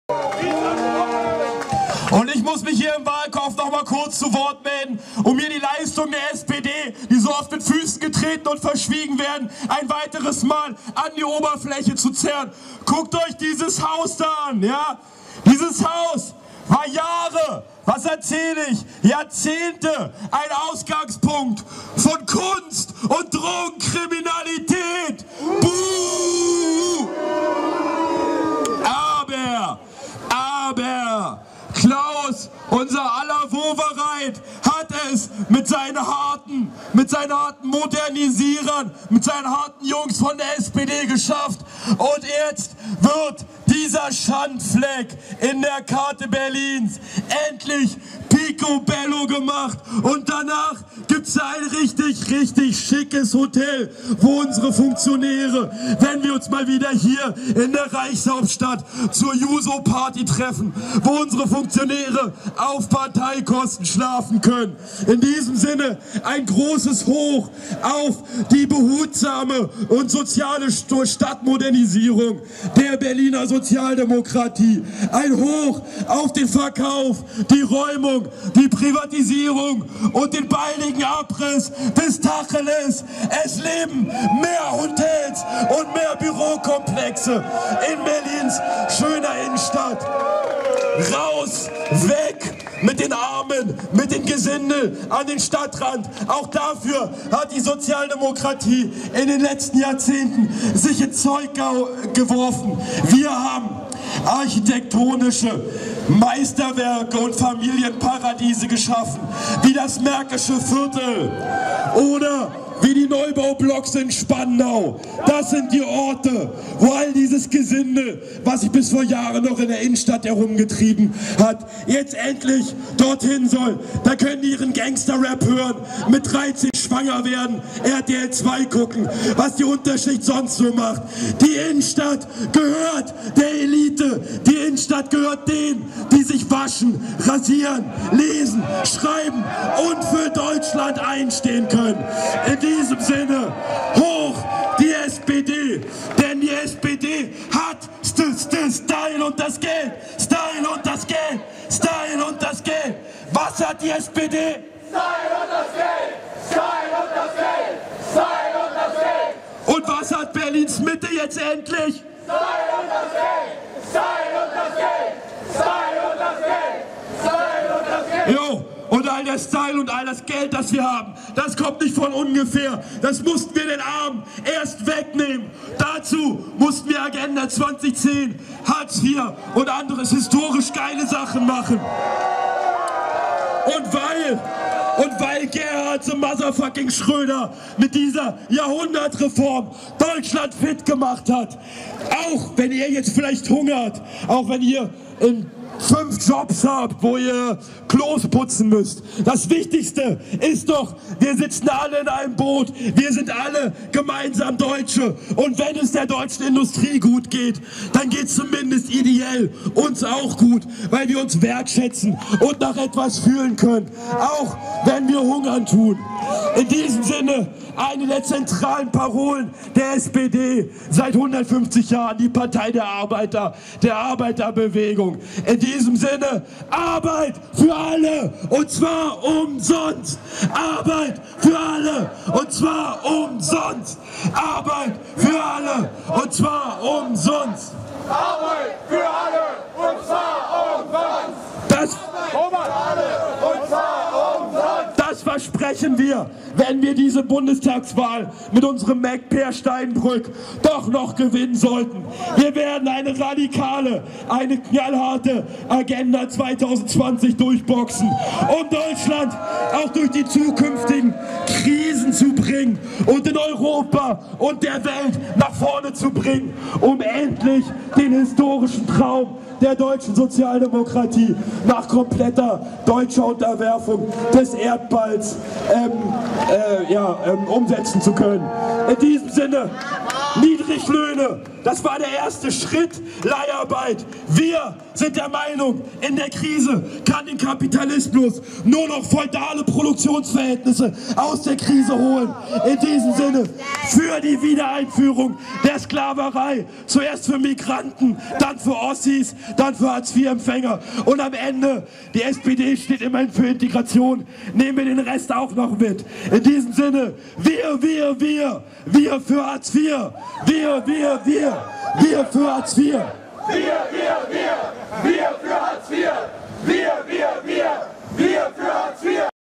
150 Jahre SPD Part013
Am 17. August 2013 feiert die SPD ihr 150-jähriges Bestehen mit einem großen Fest am Brandenburger Tor. Erwartet werden Angela Merkel, Joachim Gauck und Schnulzen-Stars wie Sammy Deluxe, Die Prinzen, Nena. Bis zu 100.000 Gäste erwartet der Gastgeber. Es hat sich ein Freundeskreis gebildet, der dieses Jubiläum mit einer Jubel-Demo gebührend feiern will. Das Motto: »Für mehr engere Gürtel, weniger Soziale Hängematten, Aufrüstung und restriktivere Aslygesetze«